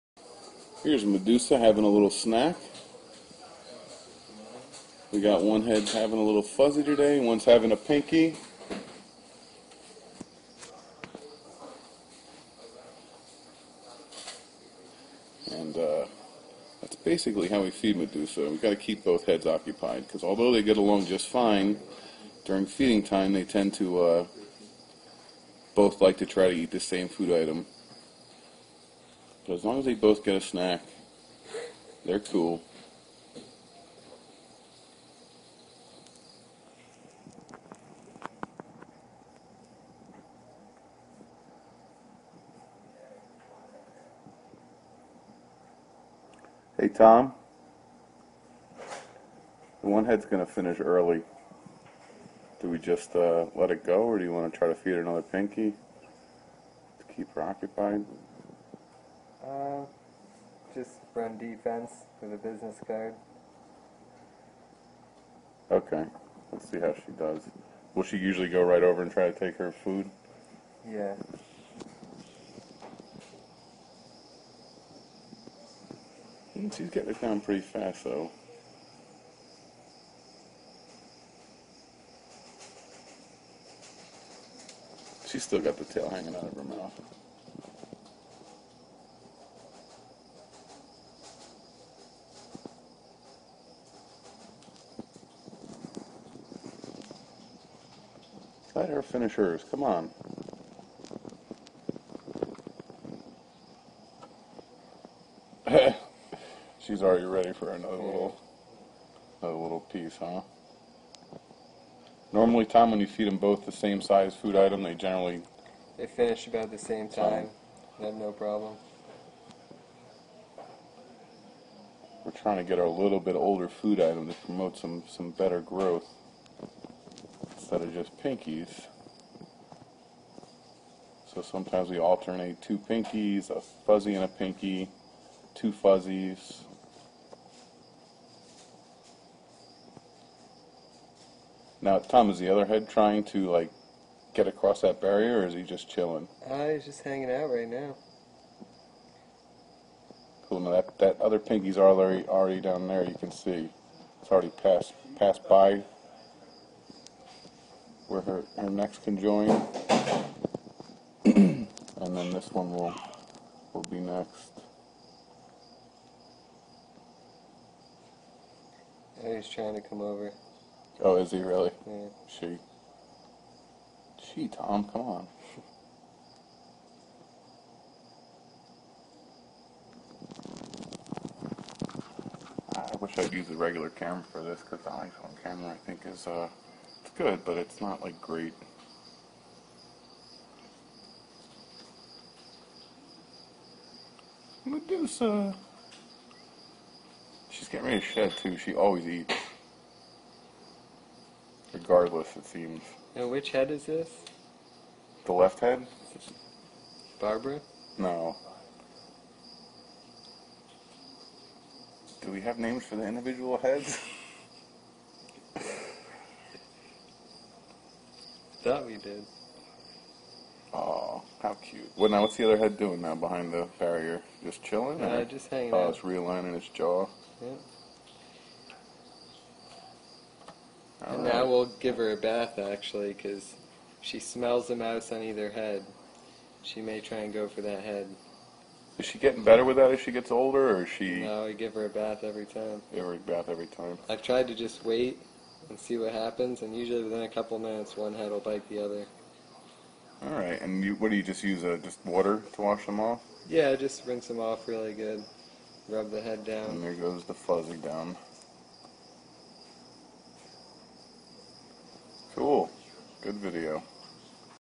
Snake Doublehead Killing Amazing/ Doppelkpöfigeschlange
Doppelkpöfige Schlange frisst Maus!! Amazing